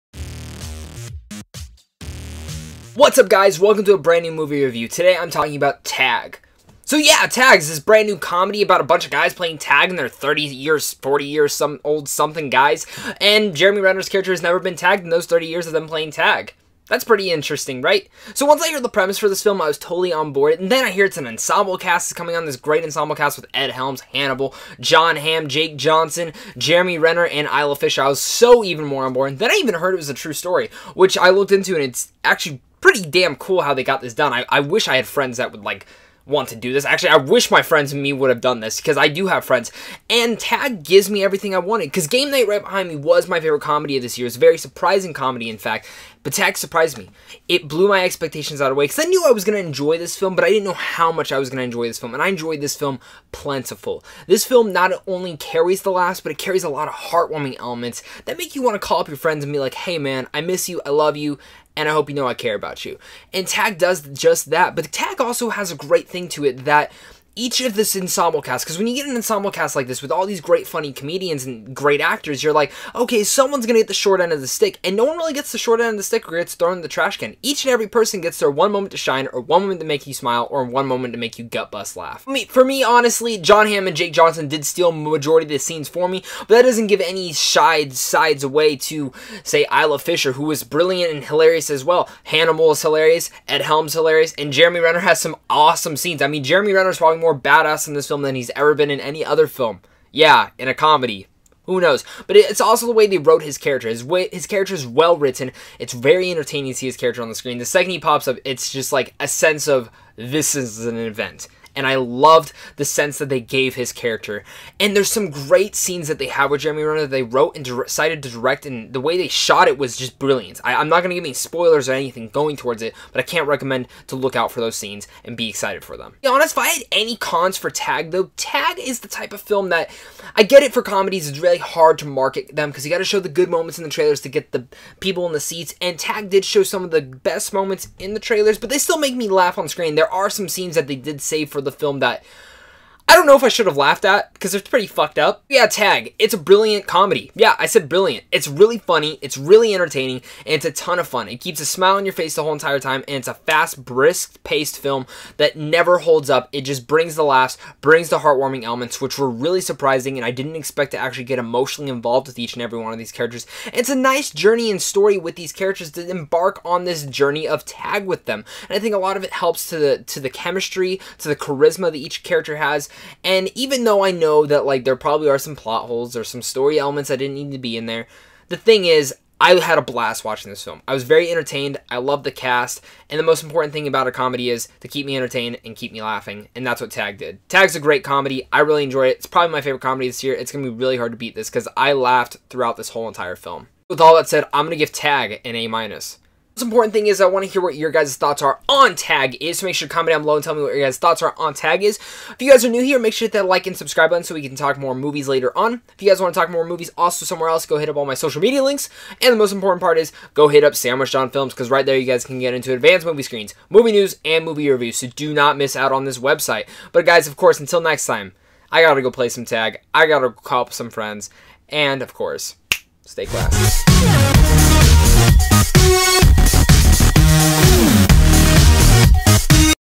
TAG - Movie Review
Synopsis: At nine years old, a group of tight-knit friends--Hoagie, Jerry, Bob, Chili and Kevin--created a game of tag that they played through the month of May. After thirty years, it was the thing that brought them together, even when their lives took them in different directions. However, this might be the last, as it seems like Jerry--who's never been tagged--might quit.

CAST AND CREW:
Directed by Jeff Tomsic
Produced by
Todd Garner
Mark Steilen
Screenplay by
Rob McKittrick
Mark Steilen
Story by Mark Steilen
Based on It Takes Planning, Caution to Avoid Being It
by Russell Adams
Starring
Ed Helms
Jake Johnson
Annabelle Wallis
Hannibal Buress
Isla Fisher
Rashida Jones
Jon Hamm
Leslie Bibb
Jeremy Renner
Cinematography Larry Blanford
Edited by Josh Crockett